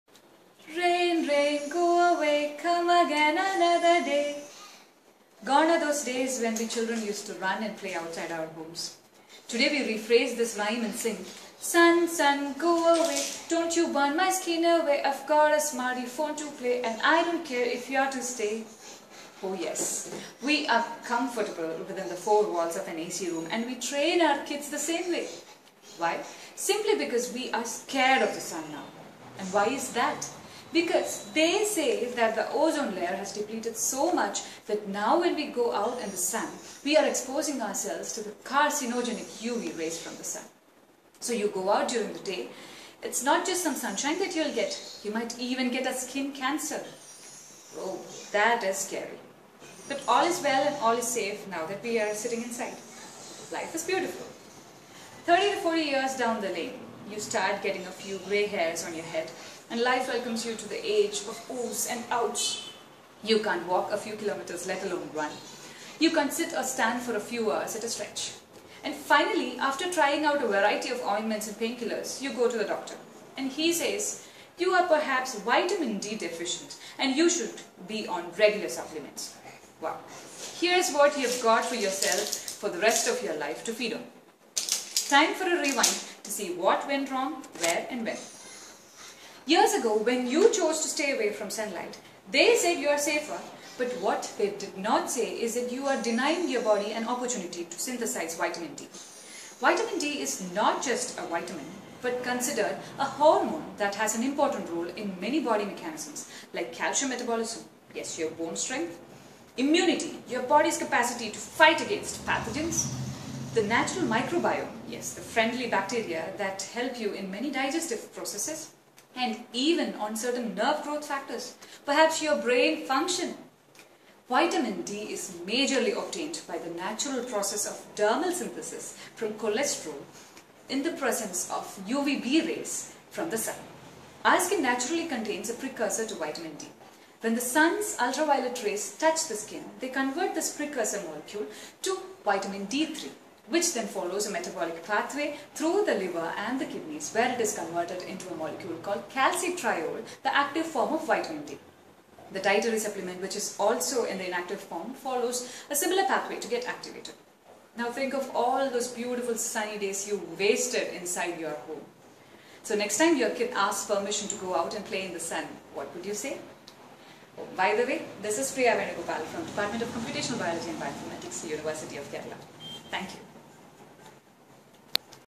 Famelab_ PriyaVenugopal_Vitamin D and Sunlight
A home-video on the importance of Sunlight and Vitamin D, prepared as part of the Famelab India 2017 contest.